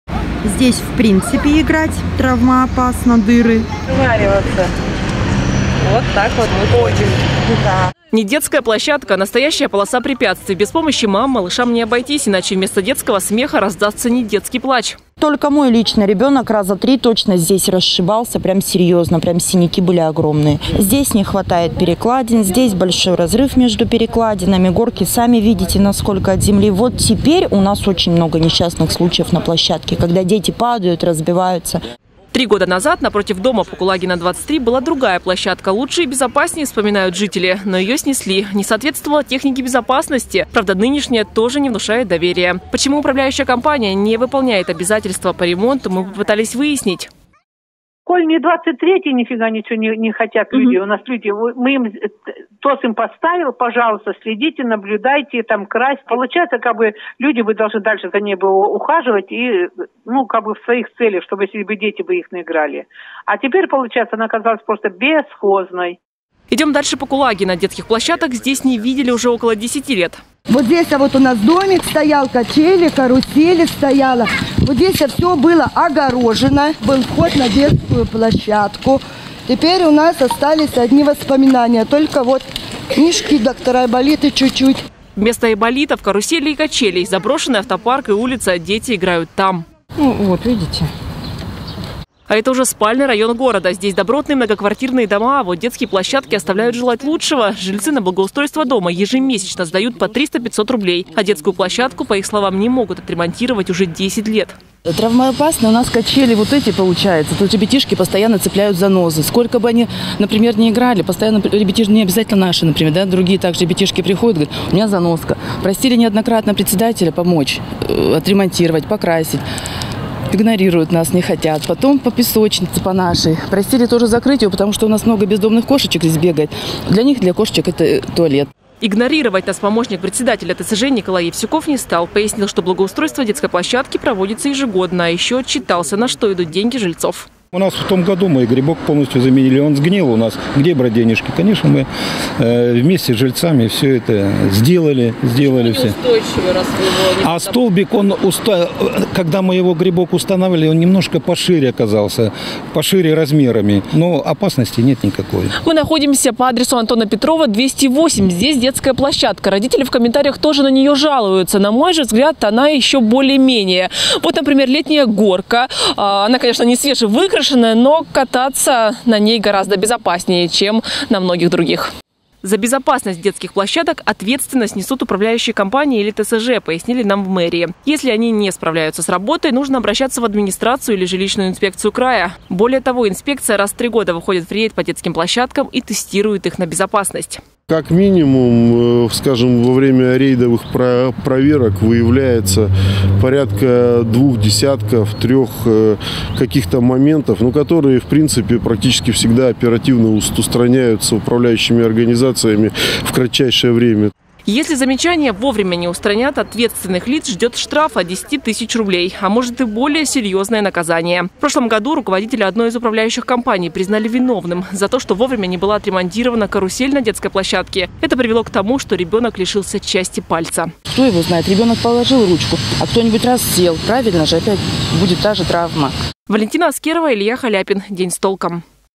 Убитые и опасные: детские площадки Барнаула вызывают опасения у родителей
Родители малышей не раз обращались в управляющие компании с просьбой решить проблему, но обратной связи так и не получили. Наша съёмочная группа отправилась в рейд, посмотреть много ли в городе опасных горок и качелей.